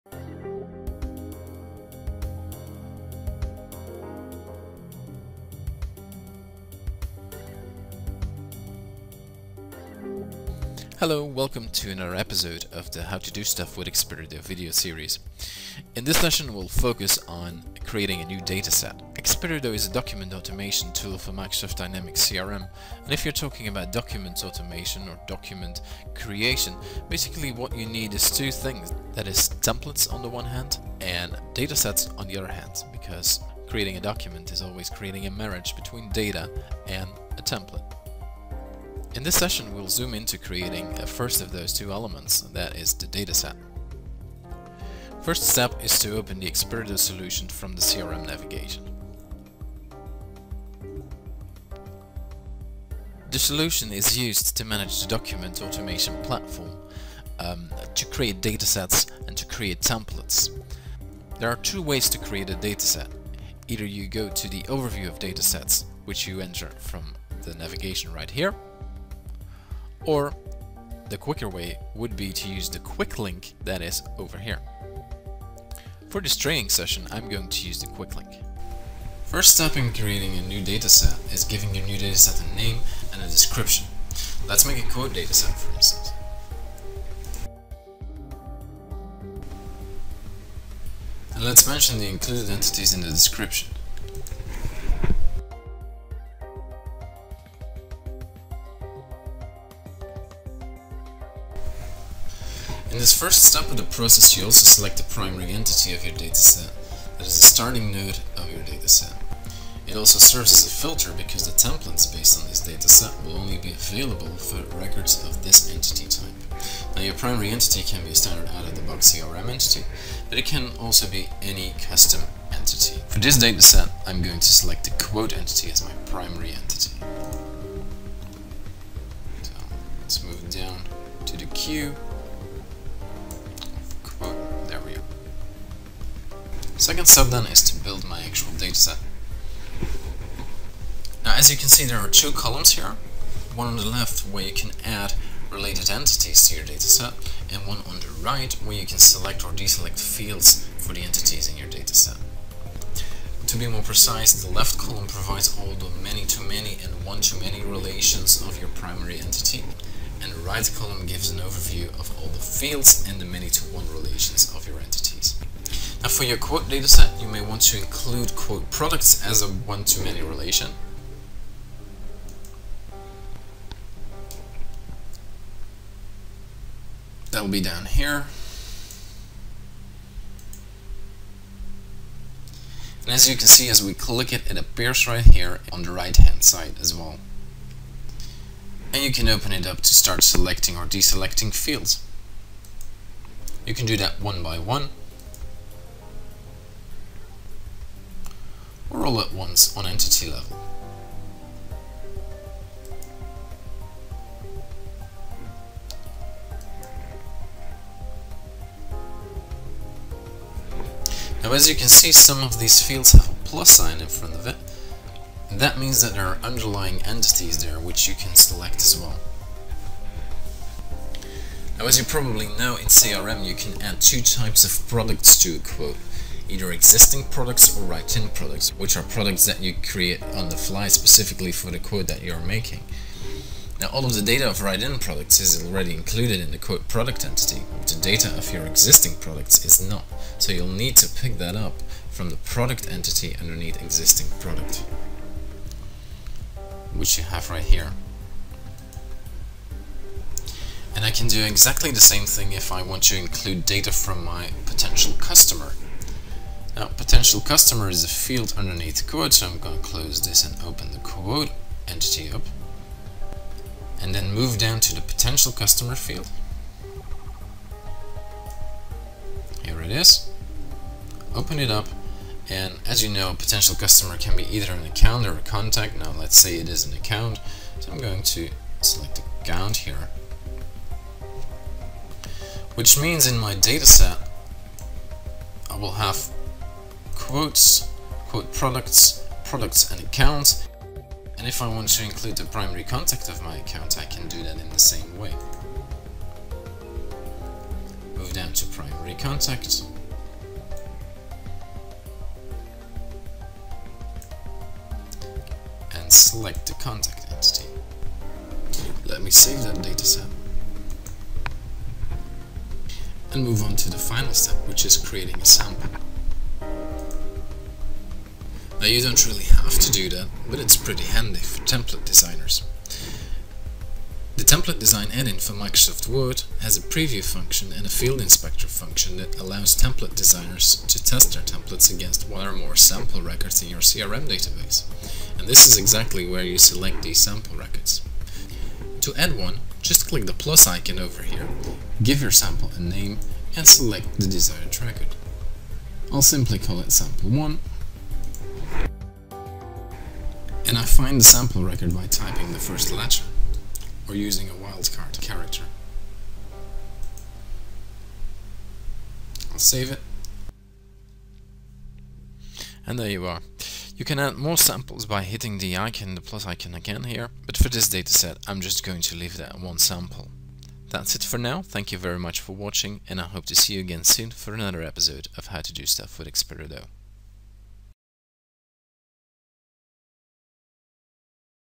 How to do stuff with XperiDo? - How to create a data set with XperiDo for Microsoft Dynamics CRM
This episode of the How to do stuff with XperiDo video series explains how you can create a data set with XperiDo for Microsoft Dynamics CRM